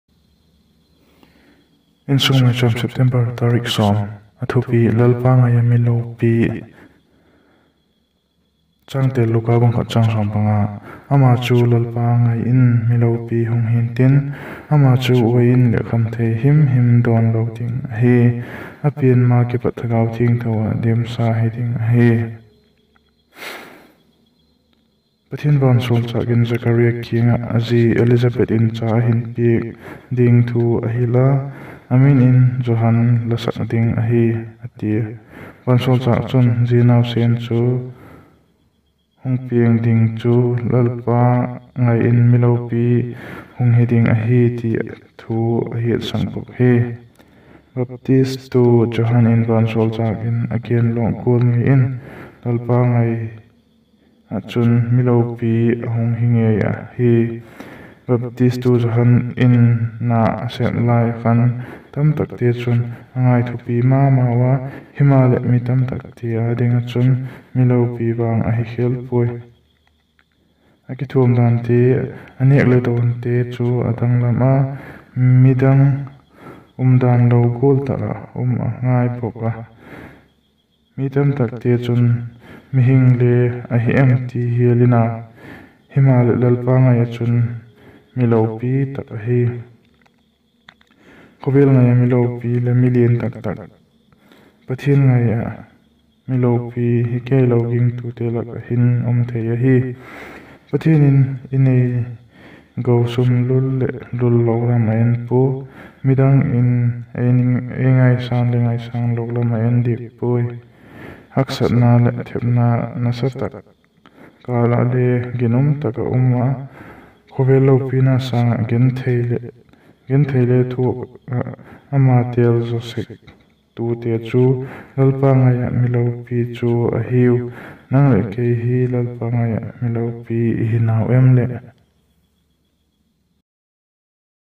LALPA NGAI A MI LIAN (LOPI) || Insung Maicham / 10th September
Ama chu Lalpa ngai in mi lopi hung hi’ntin, ama chun uain leh khamthei him him a dawn lo ding ahi; a pianma kipat Thagau Thiangtho ah dimsa hiding ahi.

InsungMaicham
INSUNG MAICHAM
Ziktu: Rev C Lalngaihawma

Zia thu hi nisin INSUNG mite leh dmani mimal thagaulam a I
khanjel theina ding ah bawl ahi.,,

Khawvel hung khangtou jel in, mihing ten hun I nei tawm tau a. Lekha simlam sang in, thu ngaithak lam nuam isa
zaw tau a ziak a chun zia lekhabu hi simthei in um ziang maleh ngaithak thei ding in ka hun guanggal ahi.

Vaiphei pau ngei a sim thei ding a ei zik piak tu Rev C Lalngaihawma kiang in lunggil tak in kipak thu I gen ahi.
Lalpan ama pui jel ta hen.
Ngaitha tu zosia Lalpan tamtak in hun malsawm tahen. Amen!